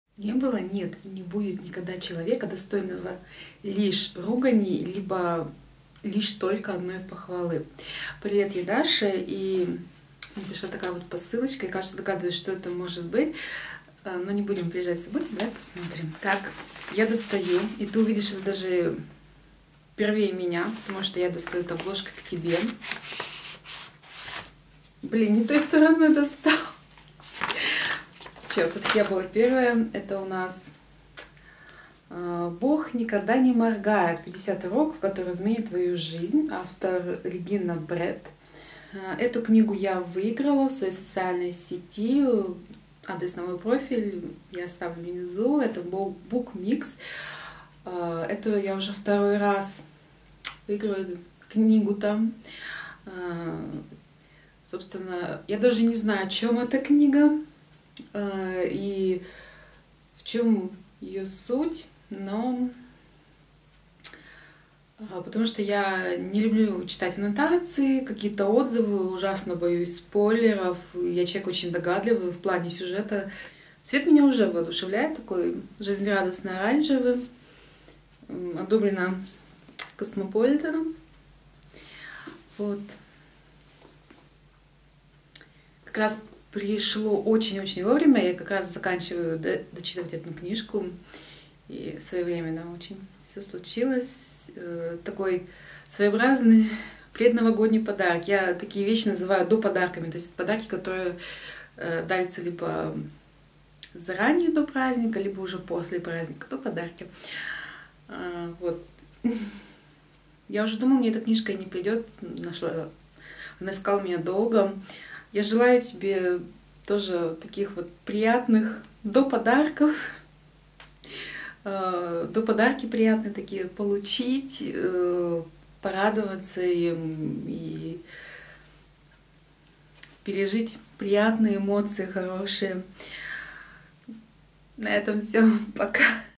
Спасибо, Bookmix!!!
И вновь мне повезло на раздаче книг! Делюсь своей радостью )))